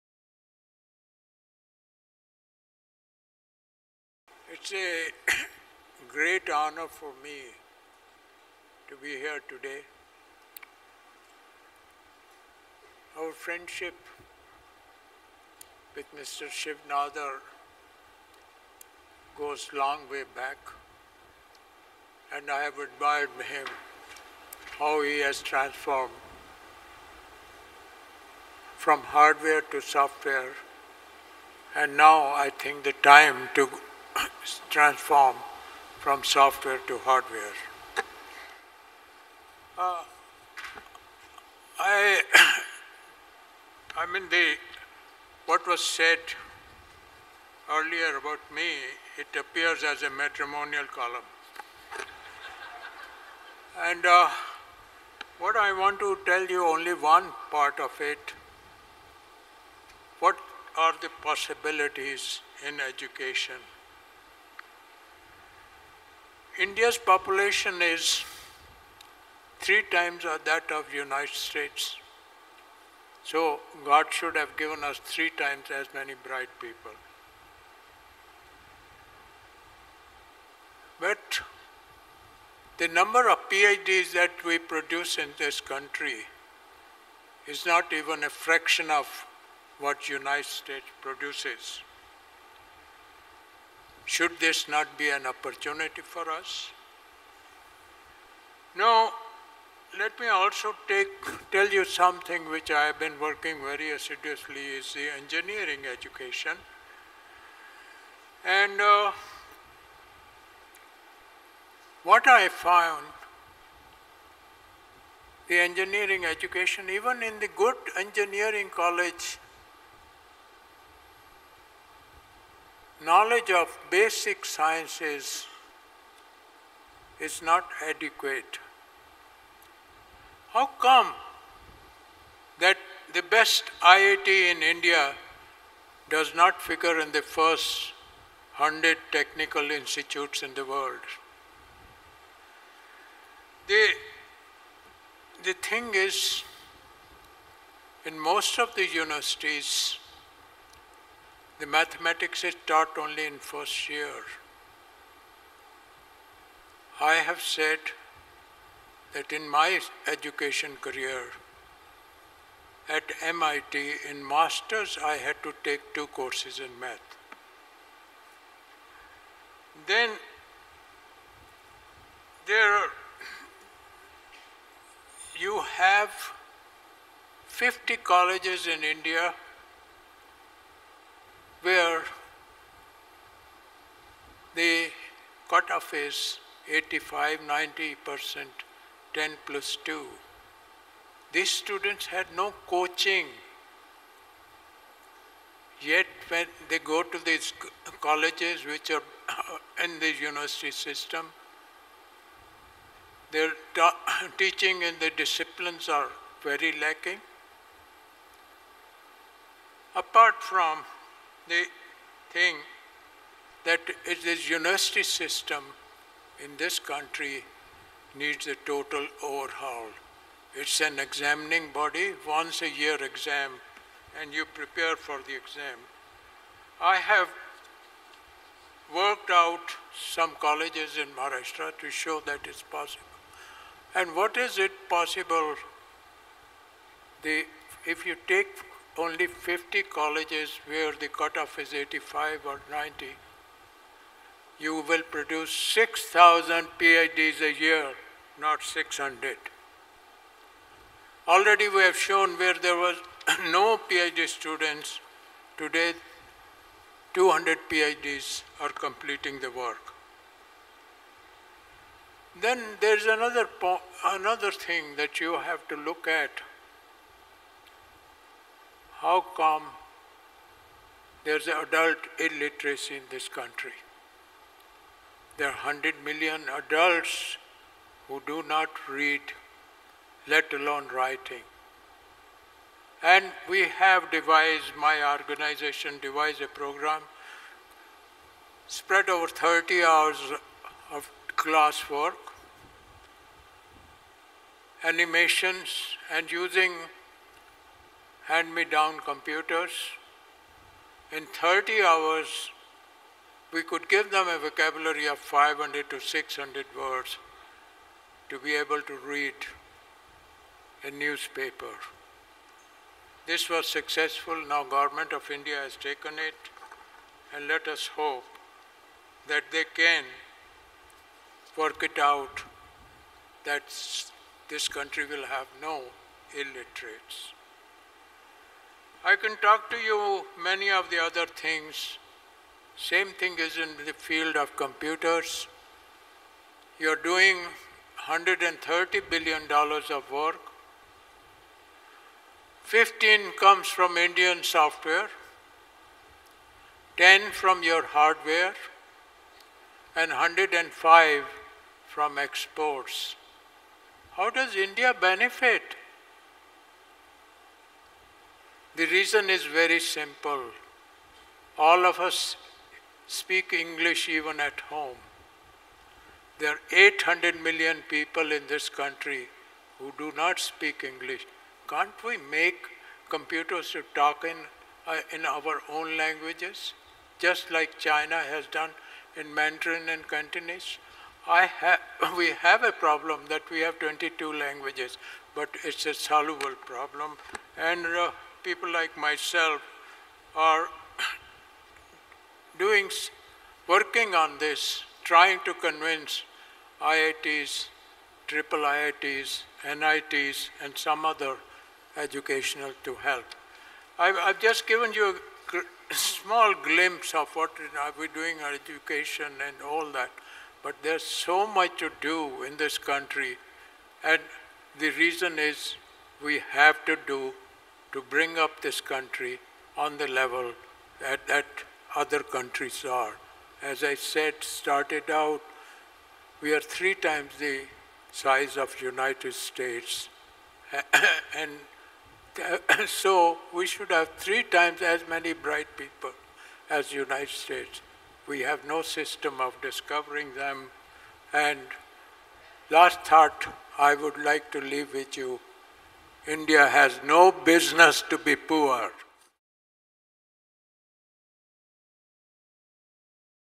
F C Kohli Founding CEO of TCS
Dr. F. C. Kohli, Founding CEO of TCS Honorary Degree Recipient

Shiv Nadar University’s Convocation Ceremony-  October 31, 2015